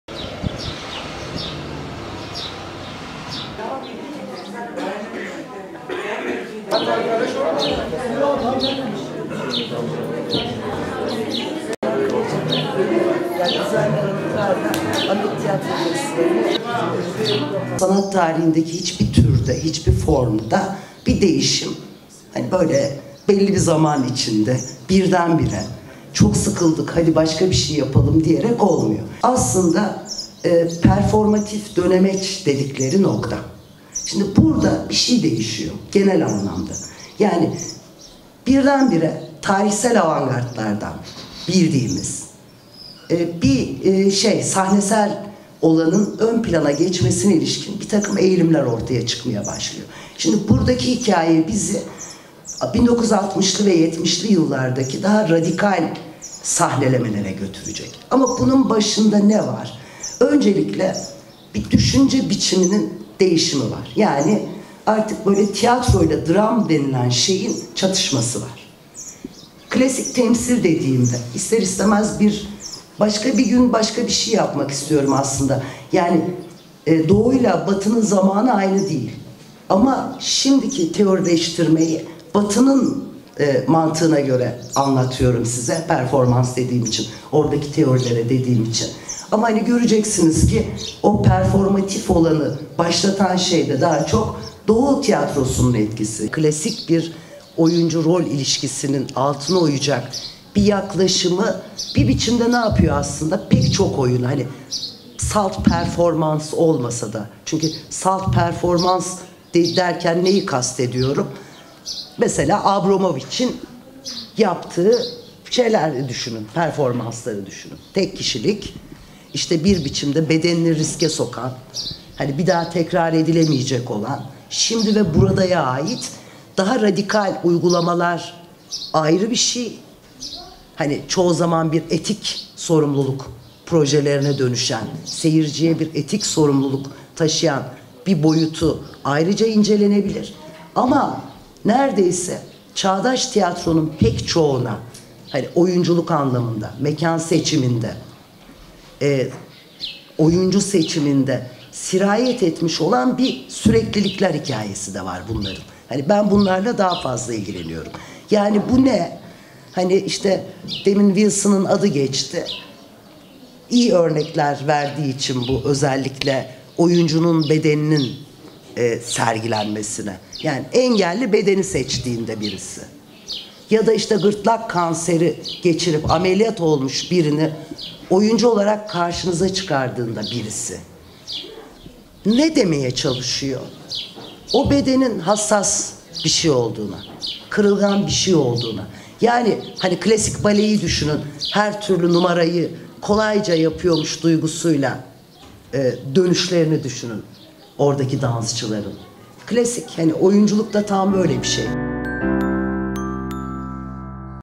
AMED TİYATRO FESTİVALİ'NDE 'TİYATRODA DEĞİŞİMLERİN TARİHİ' SÖYLEŞİSİ
Amed Tiyatro Festivali kapsamında "Tiyatroda değişimlerin tarihi" söyleşisi gerçekleştirildi. Söyleşide Doç. Dr. Yazar Süreyya Karacabey, tiyatronun tarihsel değişimine dikkat çekti.